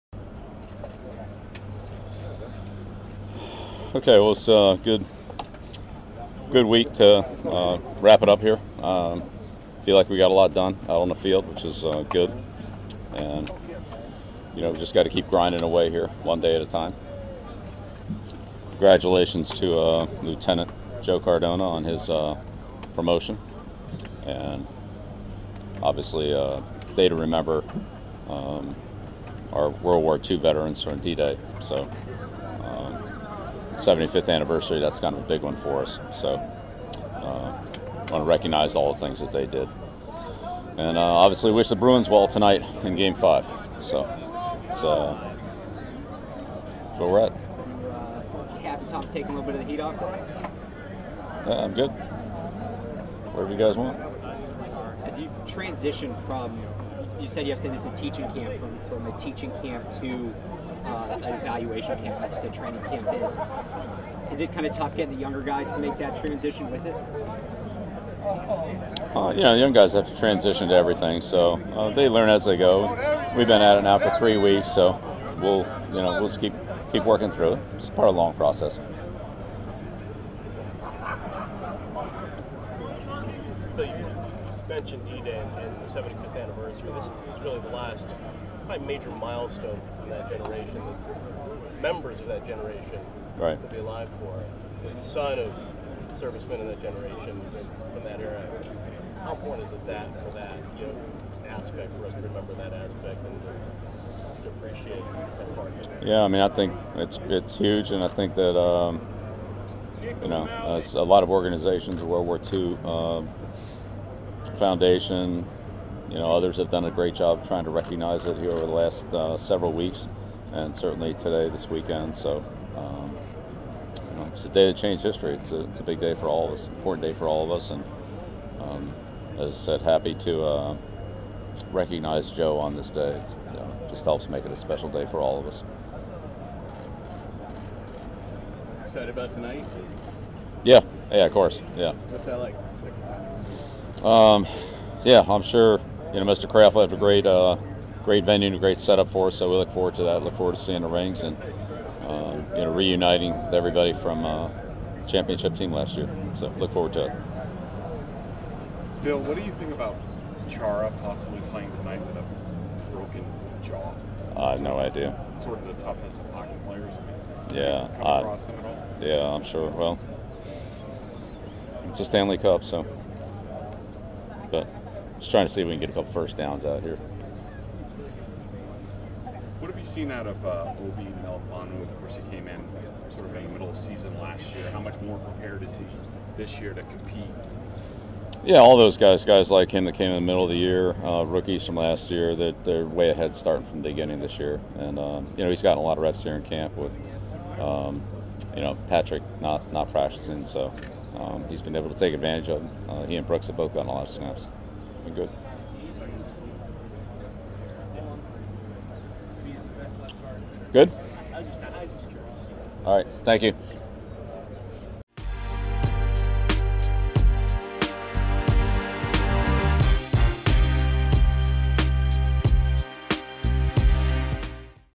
Bill Belichick: "We've just got to keep grinding away"
Bill Belichick addressed the media during day 3 of mini camp to discuss  the Bruins, the teams progress during mini camp & more.